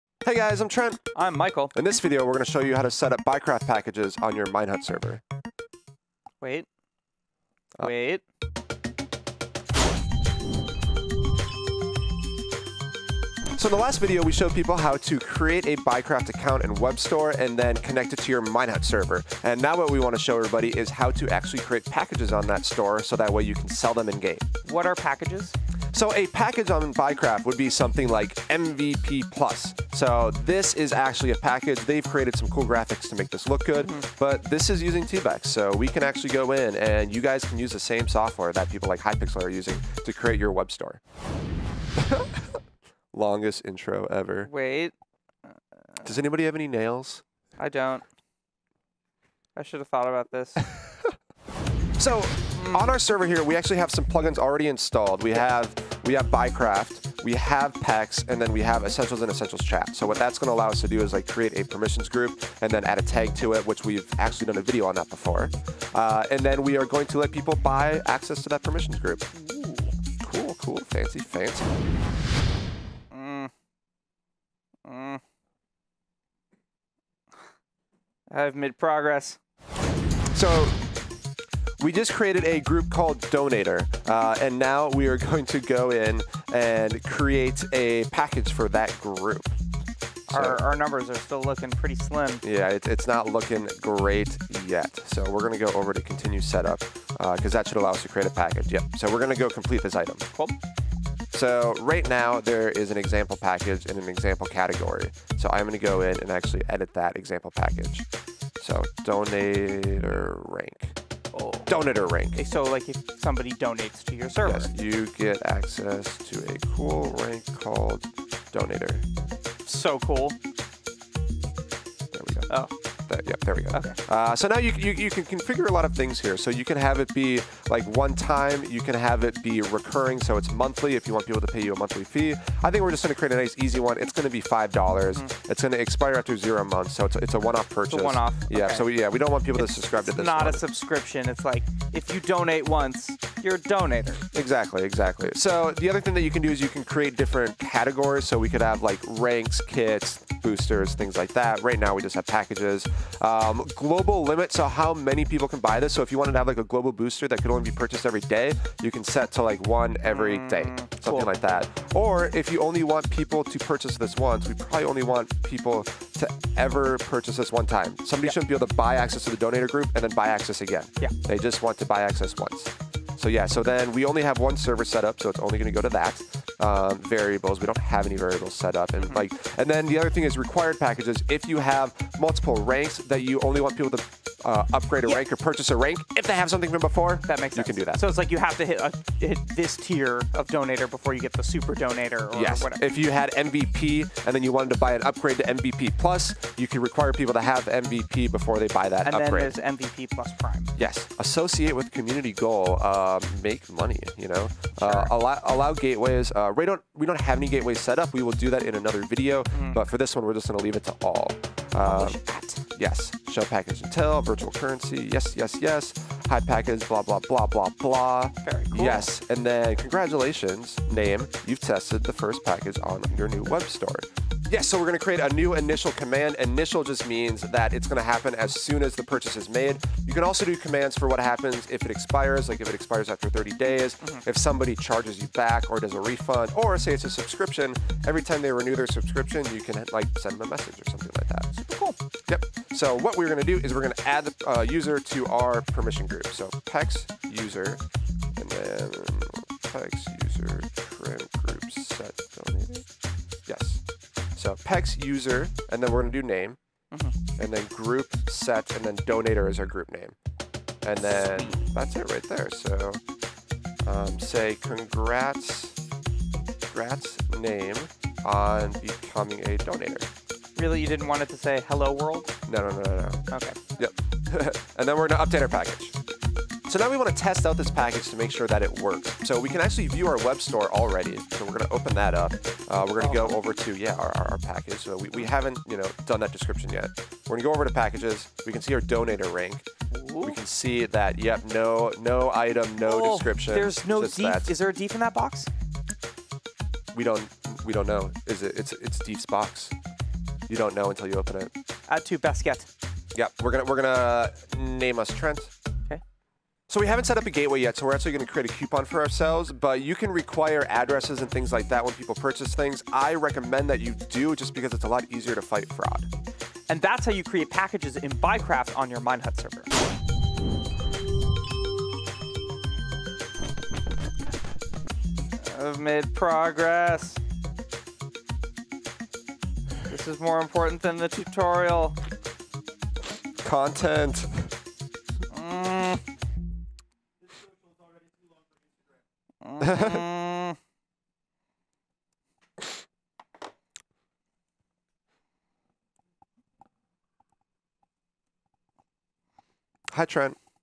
Setting Up Packages With Tebex | Minehut 101
Tebex was formerly Buycraft, so thats why they keep saying it that why in the video.